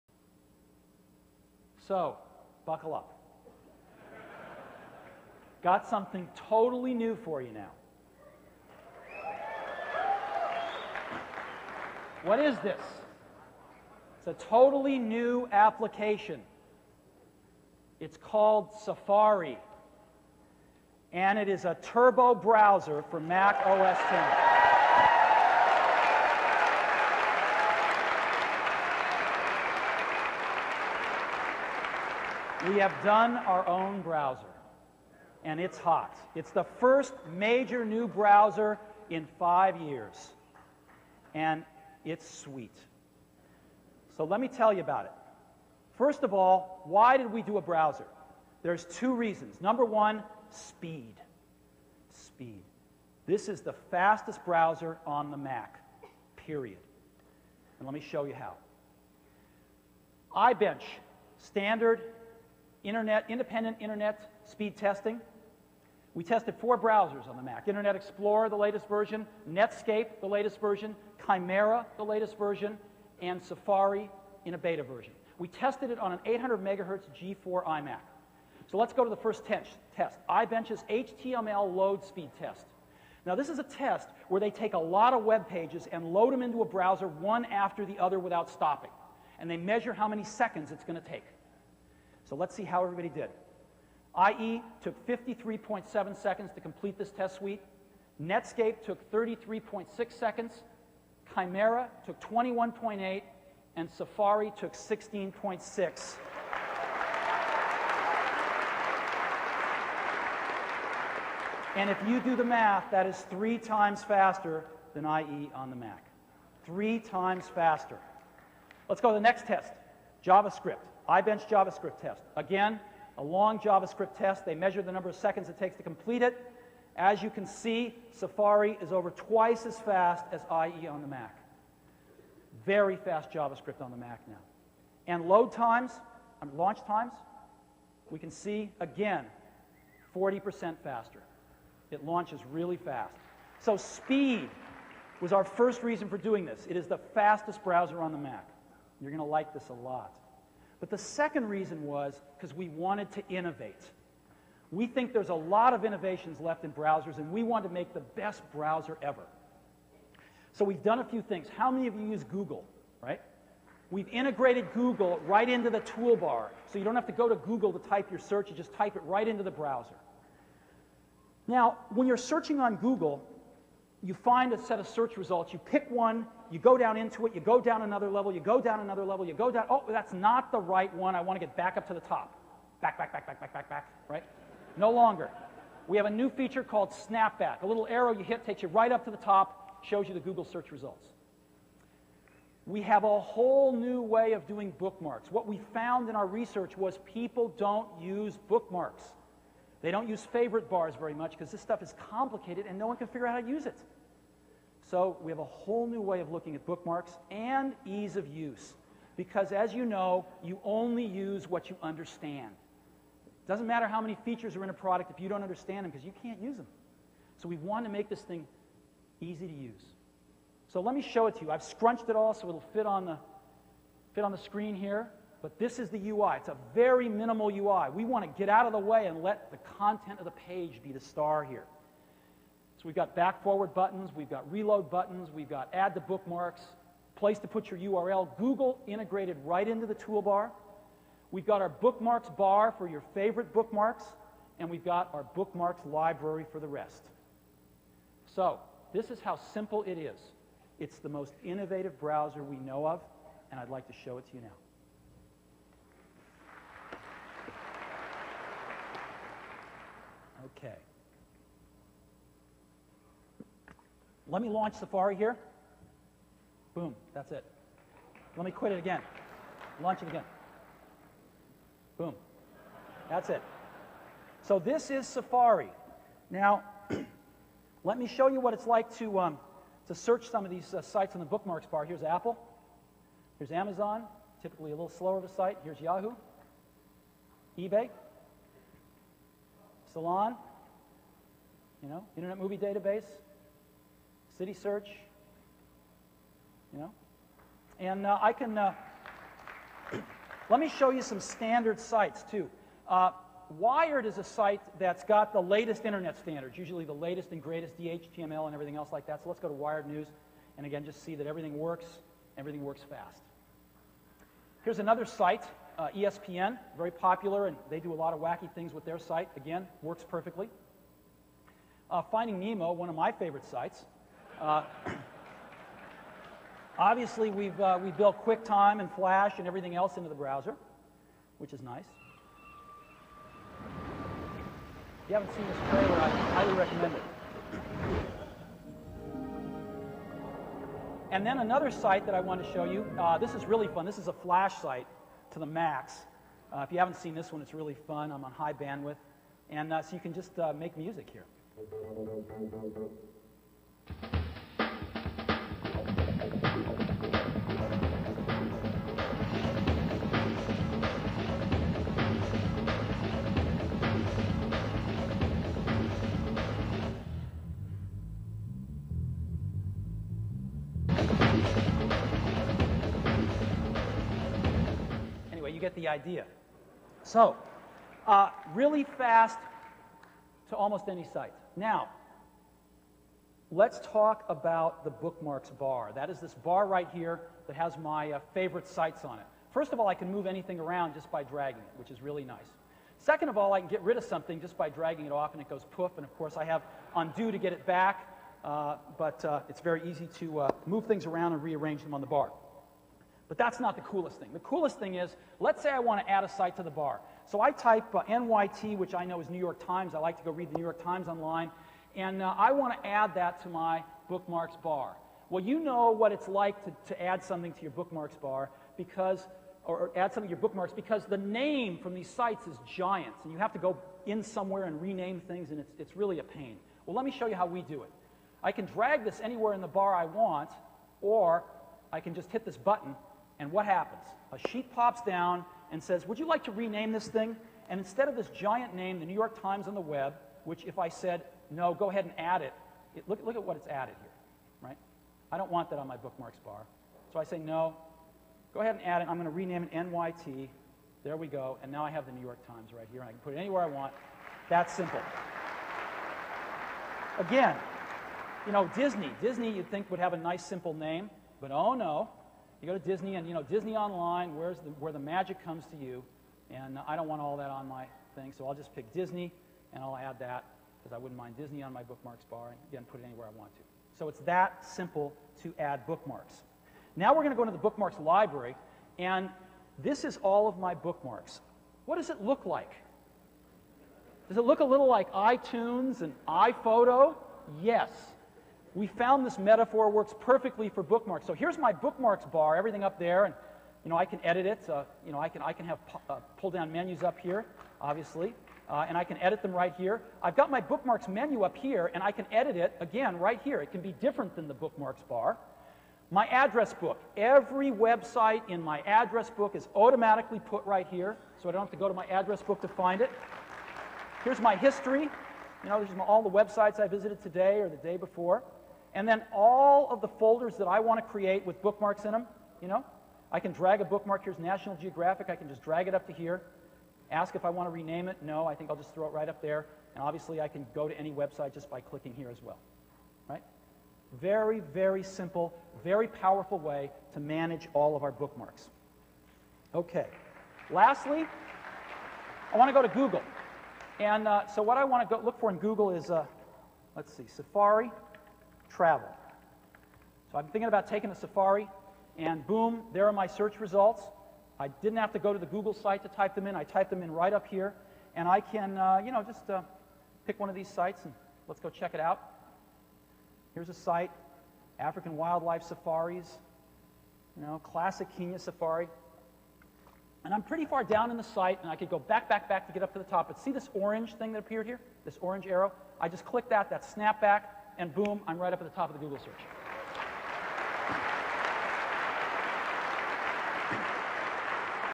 Macworld San Francisco 2003-Safari Web Browser Introduction
Here we see Steve Jobs introducing the long rumored Apple web browser, Safari.